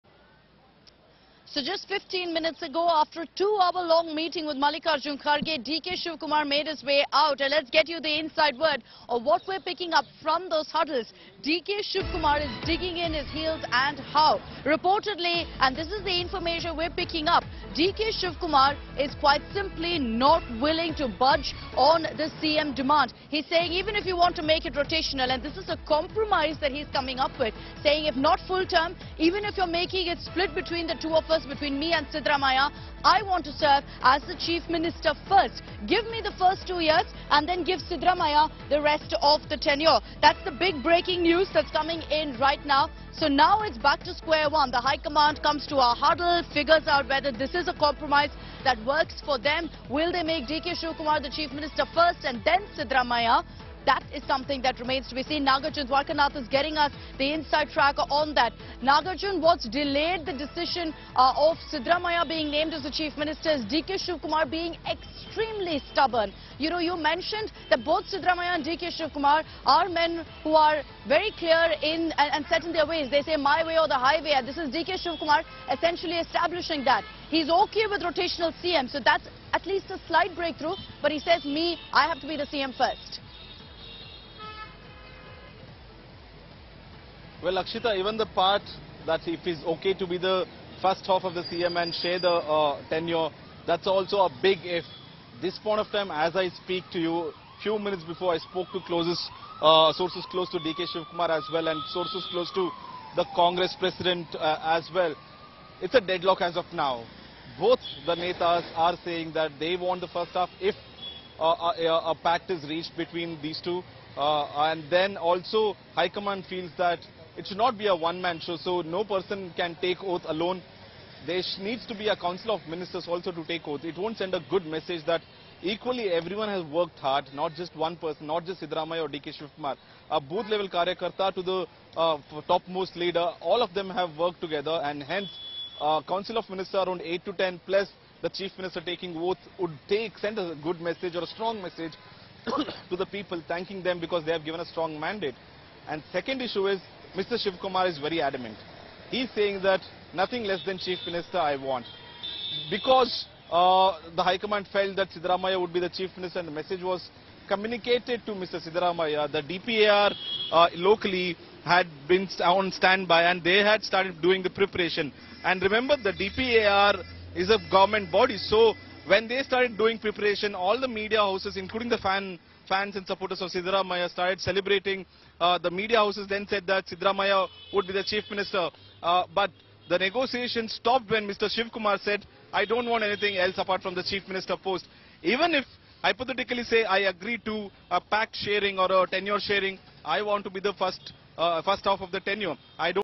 DKS Flexes Muscle, Release Photos With His MLAs Ahead Of Karnataka CM Announcement
Senior Congress leader Siddaramaiah is set to be the next Karnataka chief minister, according to sources. DK Shivakumar, another contender for the top post, will be offered the deputy chief ministerial post, according to sources. He is expected to get important portfolios in the state Cabinet, sources added. Siddaramaiah's swearing-in is likely to take place on Thursday. A meeting of all the 135 Congress MLAs is expected to be held on Thursday.